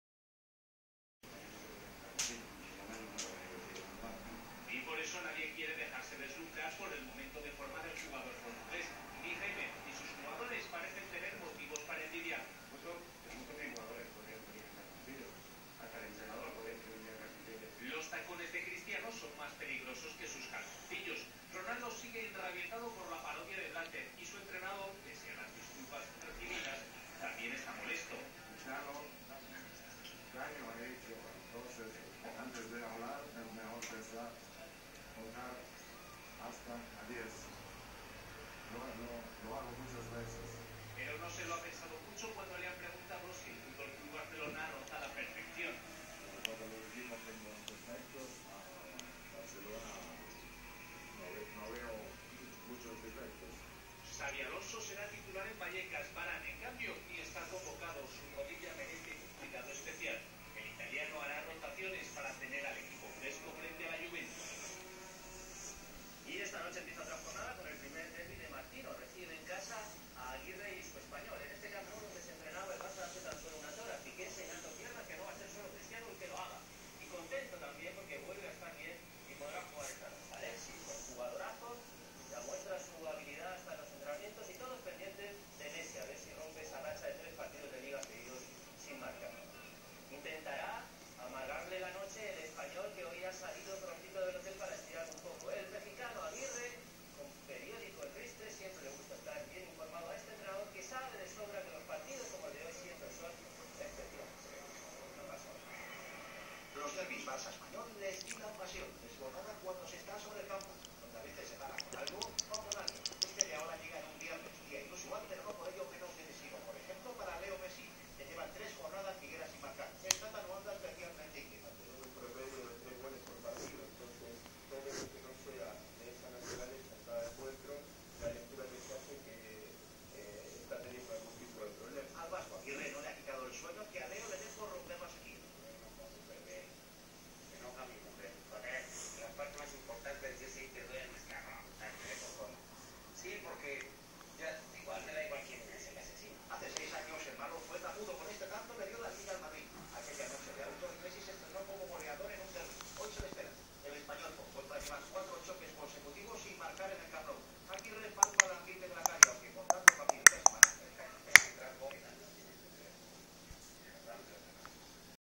FLOPO GATO GUAY
Flopo educado,le gusta la tele come a su lado,bebe agua en vaso y mas.